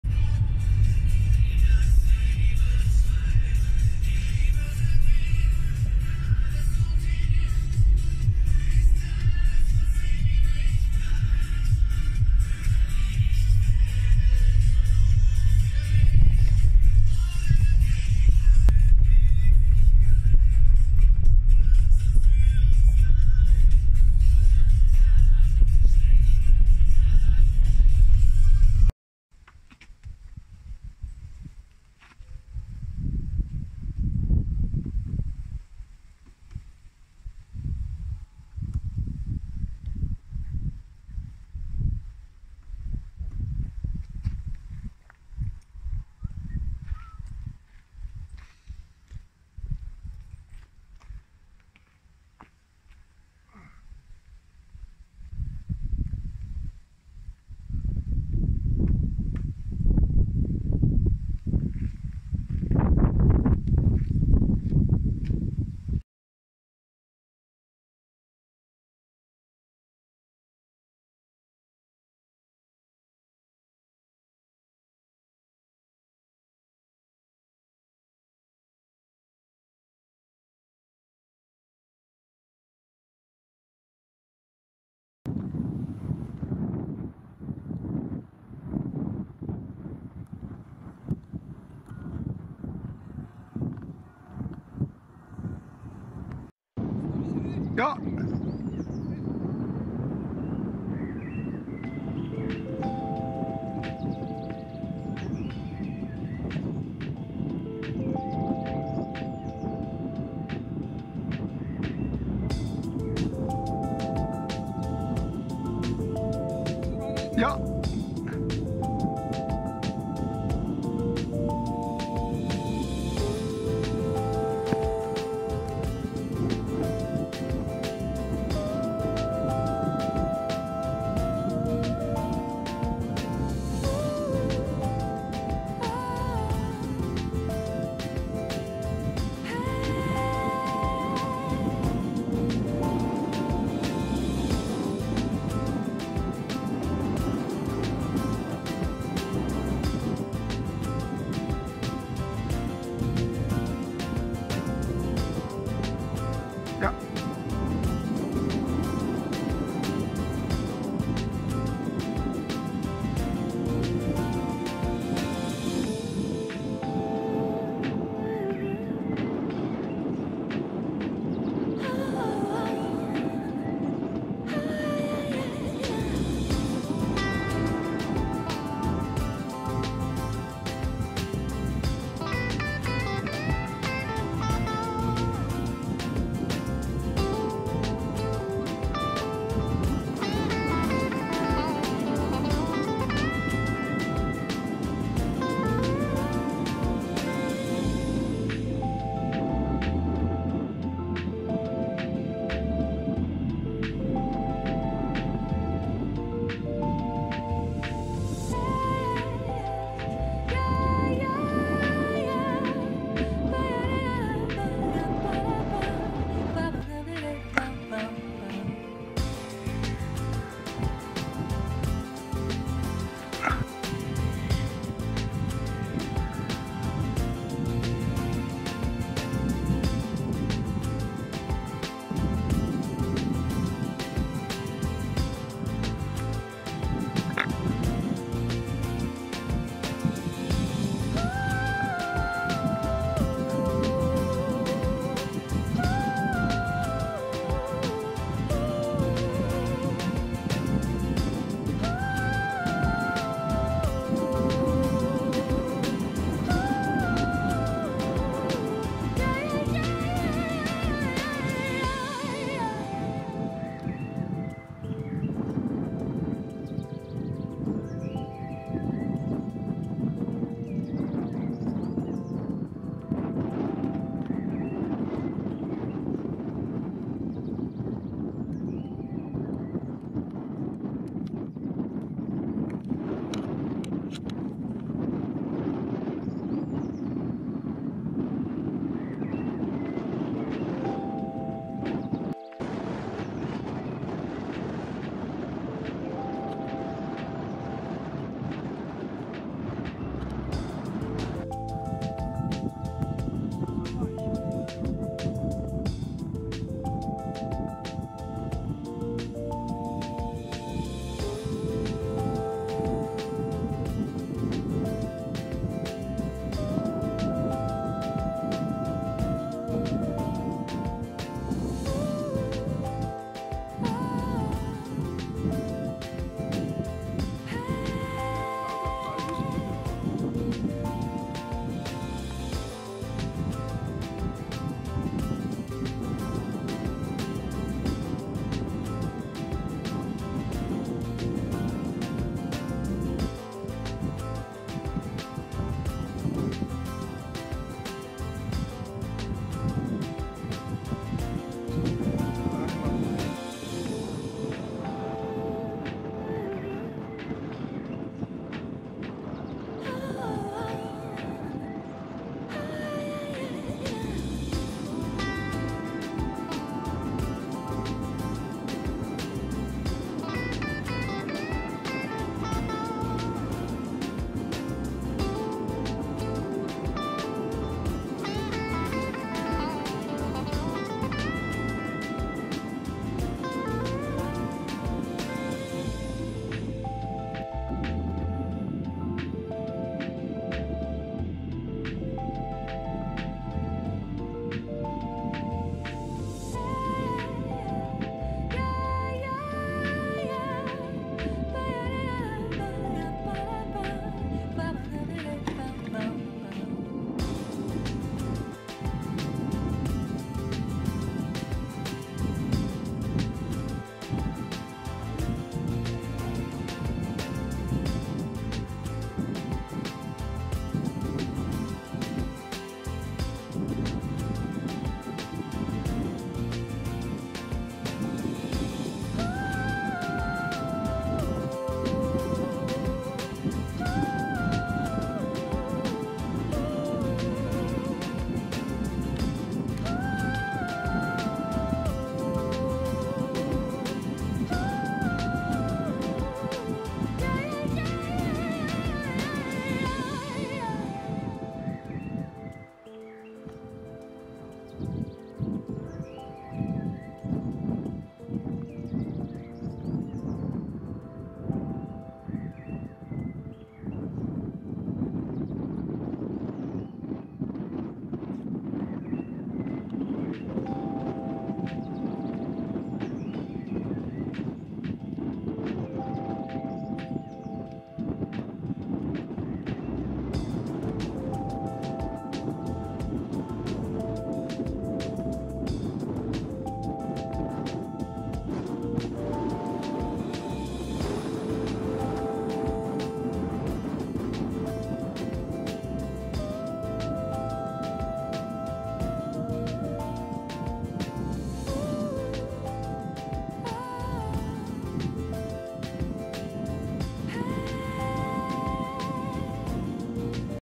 Hangflug am Hesselberg
Hangflug am Hesselberg an einem wunderschönen Oktobertag 2021, bei idealem Wind aus Süd Südwest